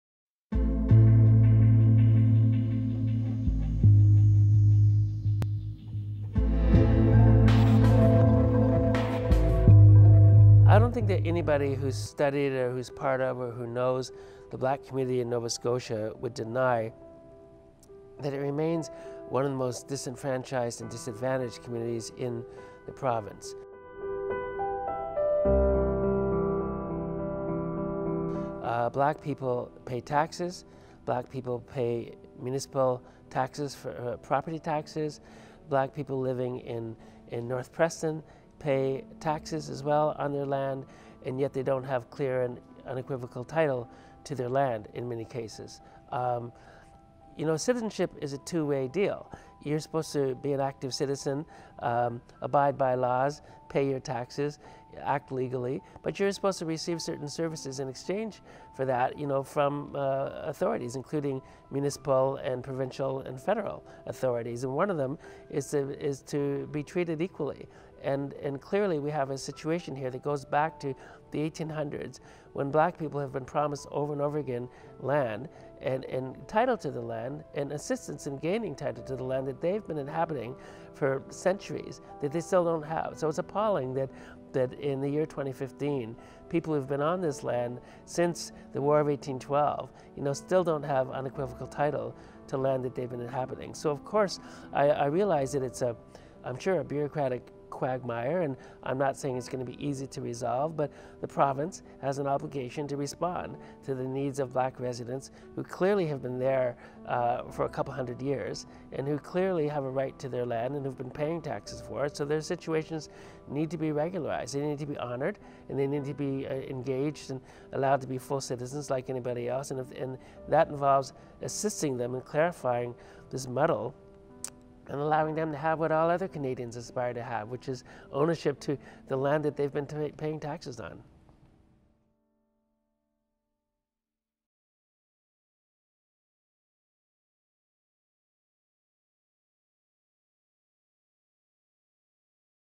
Untitled Land - Lawrence Hill
Lawrence Hill weighs in on land title issues in North Preston, Nova Scotia.